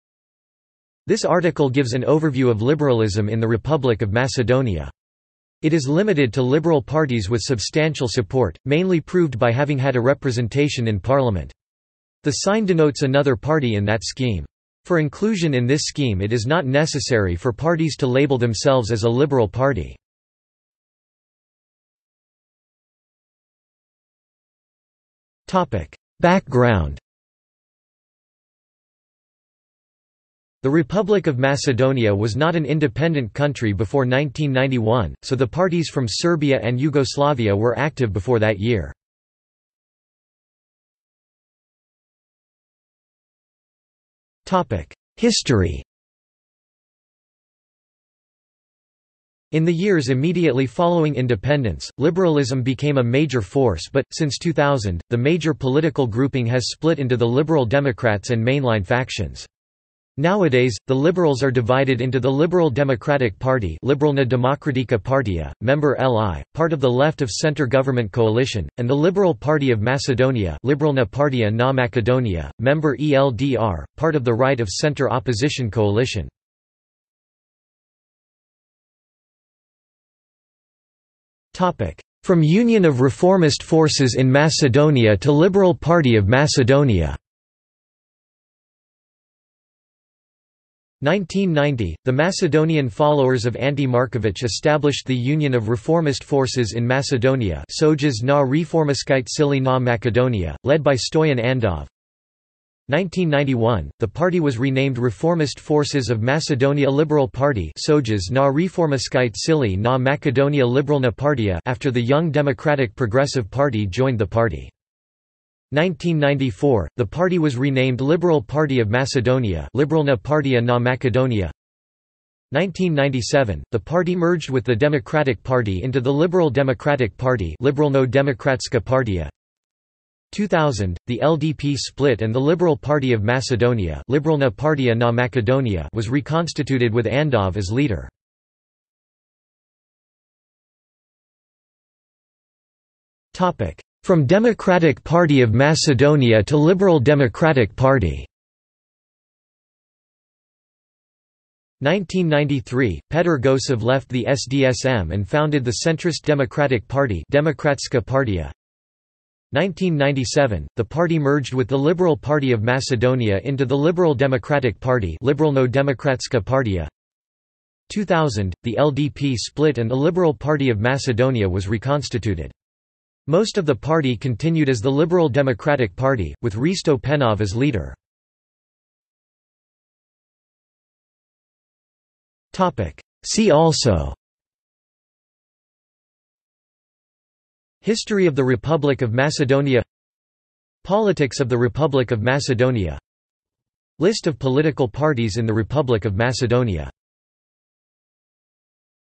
Liberalism in the Republic of Macedonia | Wikipedia audio article
This is an audio version of the Wikipedia Article:
Liberalism in the Republic of Macedonia

00:00:21 1 Background
00:00:40 2 History
00:01:18 2.1 From Union of Reformist Forces in Macedonia to Liberal Party of Macedonia
00:02:20 2.2 From Democratic Party of Macedonia to Liberal Democratic Party
00:02:58 3 See also

- Socrates

SUMMARY
This article gives an overview of liberalism in the Republic of Macedonia. It is limited to liberal parties with substantial support, mainly proved by having had a representation in parliament. The sign ⇒ denotes another party in that scheme. For inclusion in this scheme it is not necessary for parties to label themselves as a liberal party.